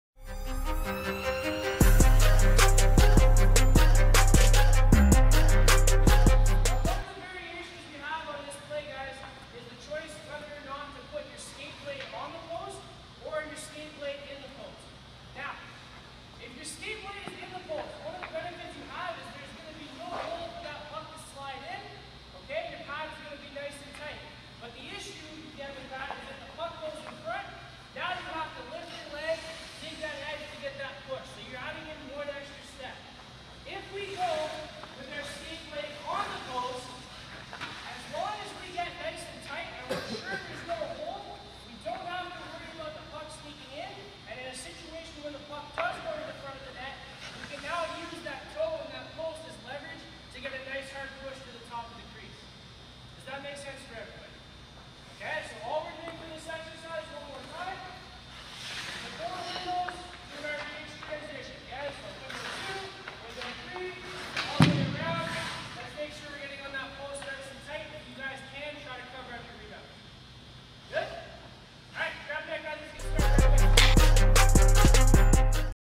Goalie Training Tips: Pad-in vs Skate on Post (RVH Applications)
There's no right or wrong way to stop pucks, but there's always a better way of becoming more efficient.

Coach Chris explains the RVH technical applications to a group of students at one of our goalie training sessions so that they may understand how and when to apply them, as well as some of their pros and cons.